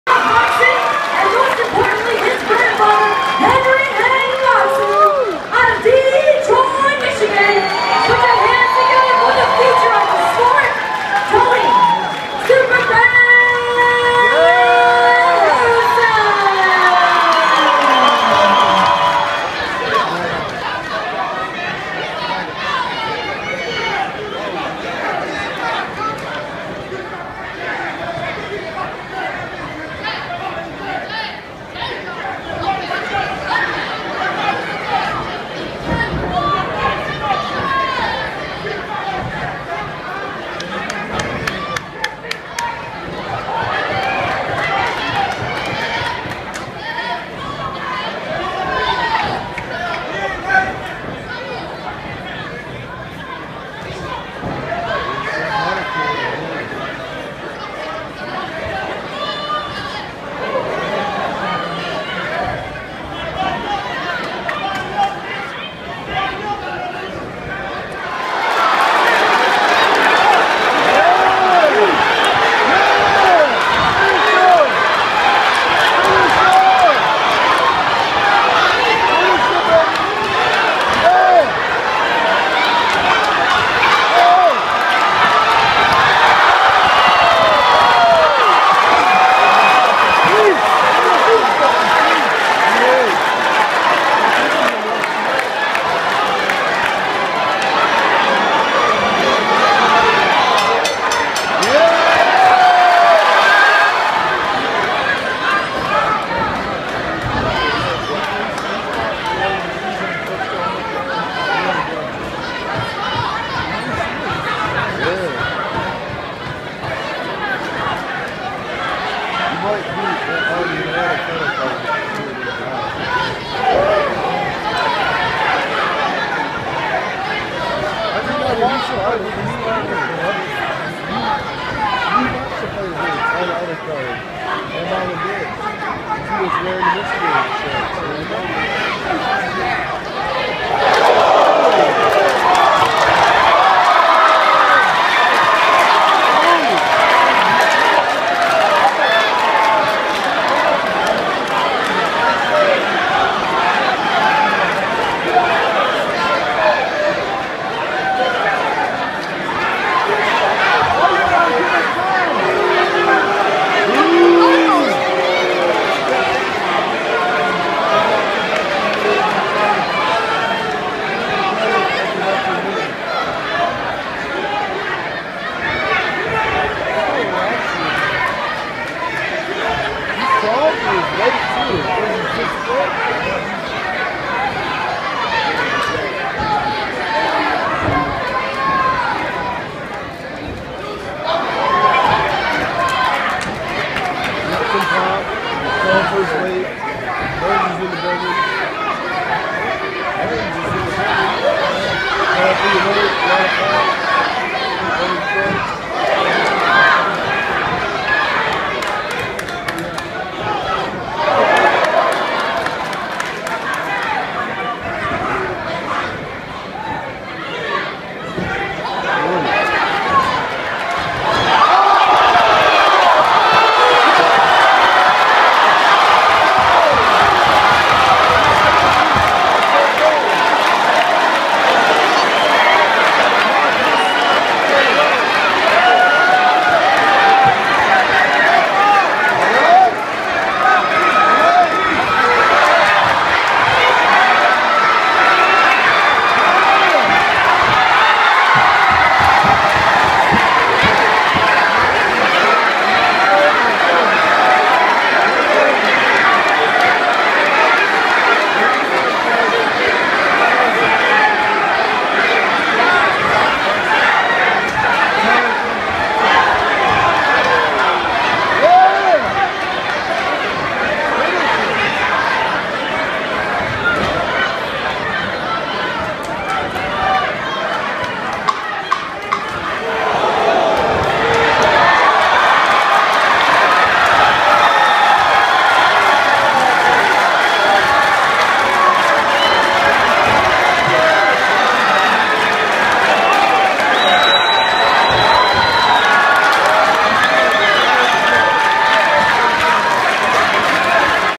Tony Harrison vs Grady Brewer
Highlights from Tony "Superbad" Harrison KO of Grady Brewer in the 2nd Round on Feb 7th at the Masonic Temple in Detroit MI.